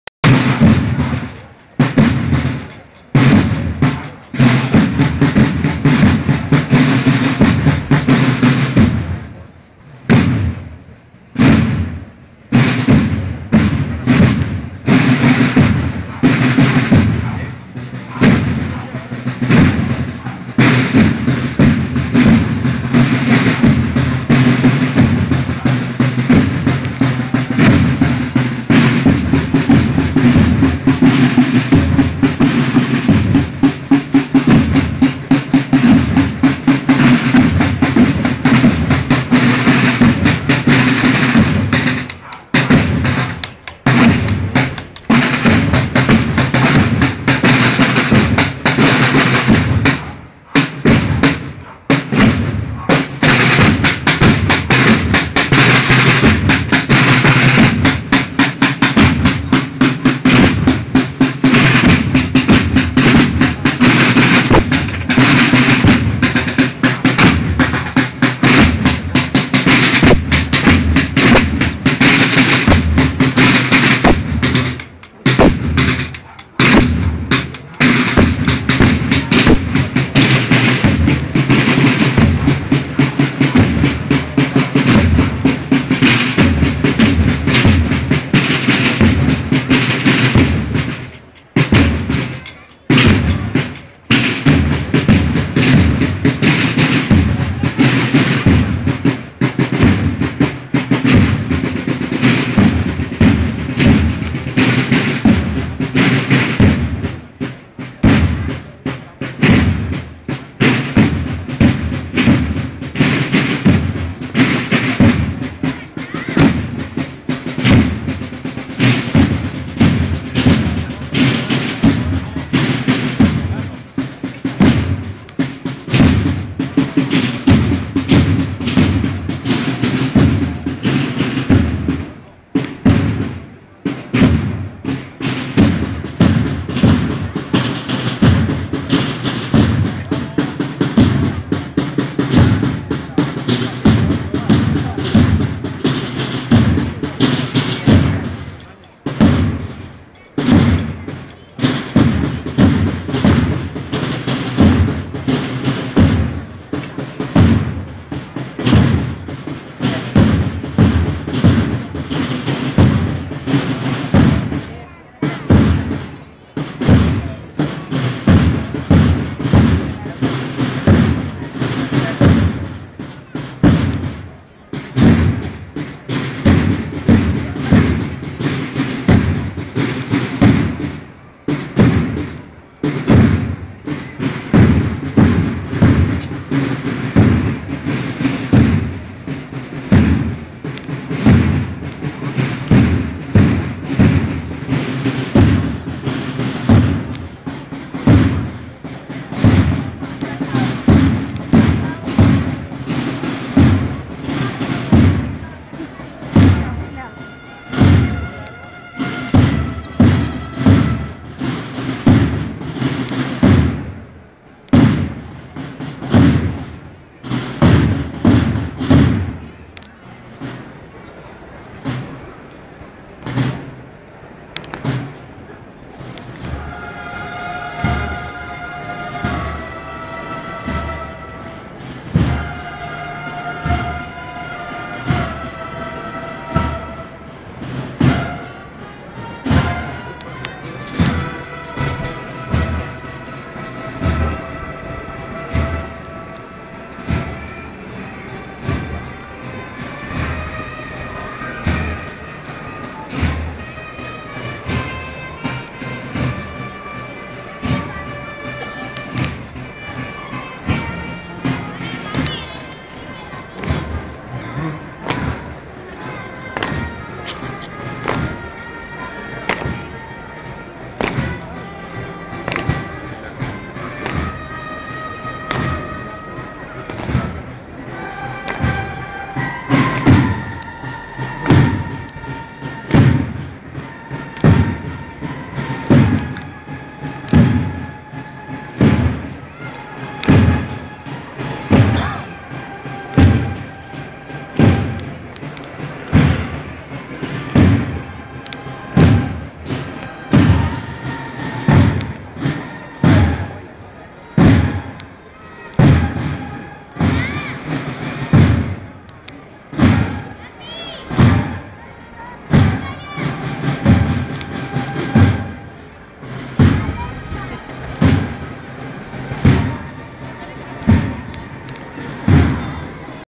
Soledad Dolorosa - Segovia (Jueves Santo).mp4
Procesión del Paso de la Soledad Dolorosa en Segovia. Esta cofradía tiene su parroquia titular en Santa Eulalia